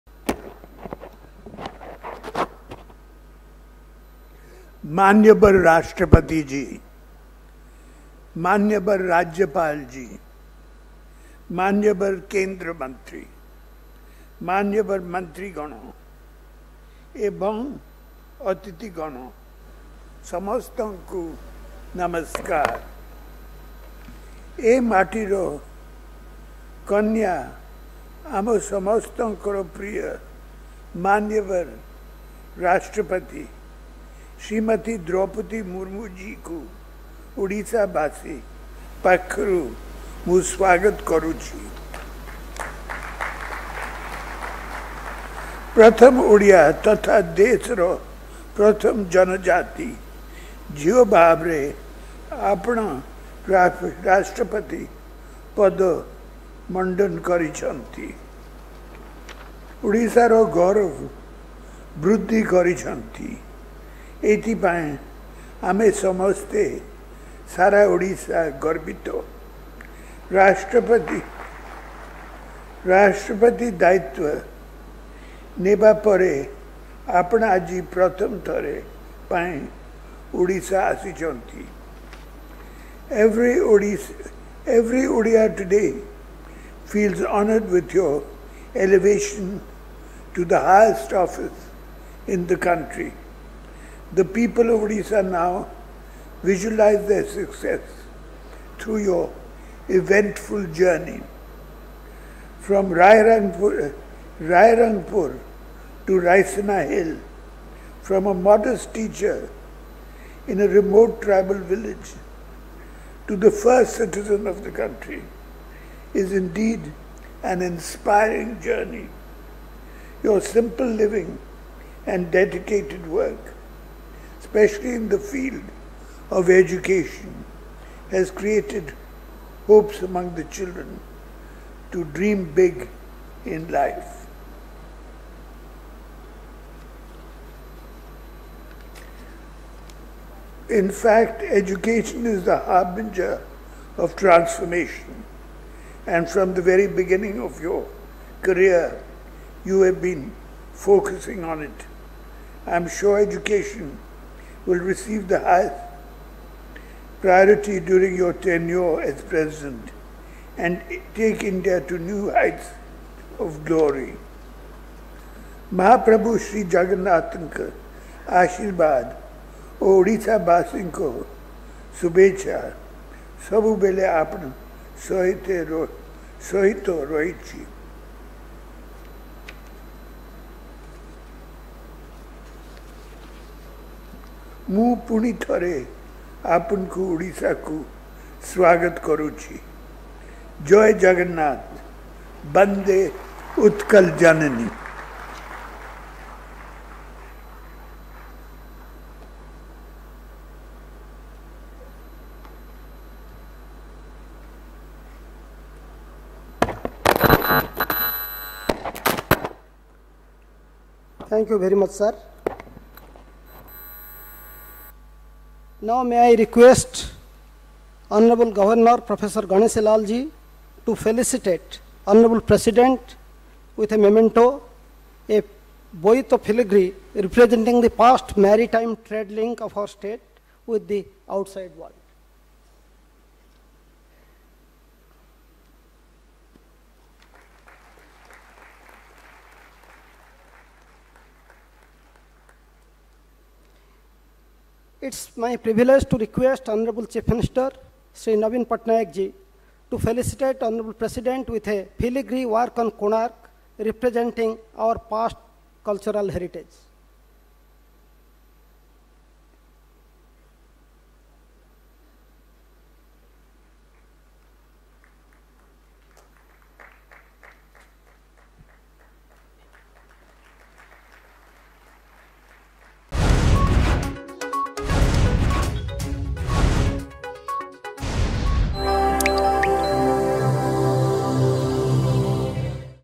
ରାଷ୍ଟ୍ରପତି ଦ୍ରୌପଦୀ ମୁର୍ମୁଙ୍କ ପାଇଁ ଆମେ ବେଶ ଗର୍ବିତ
ପ୍ରଥମ ଓଡିଆ ମହିଳା ଭାବେ ରାଷ୍ଟ୍ରପତି ହୋଇଛନ୍ତି ଦ୍ରୌପଦୀ ମୁର୍ମୁ
ଏଥିପାଇଁ ଆମେ ବେଶ ଗର୍ବିତ : ମୁଖ୍ୟମନ୍ତ୍ରୀ ନବୀନ ପଟ୍ଟନାୟକ